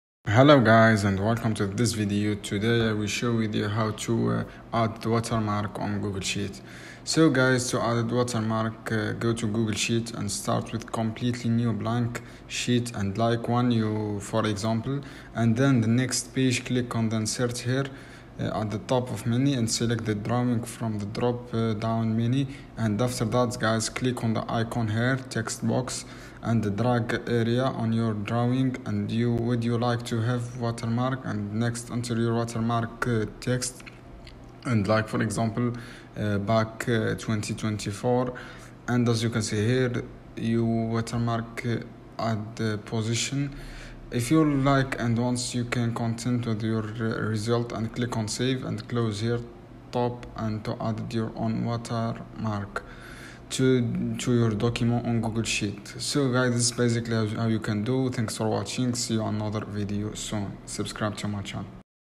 How To Add Watermark In Google Sheets - ( FULL GUIDE) 2024
in this video i will show you how to put watermark on google sheets step by step..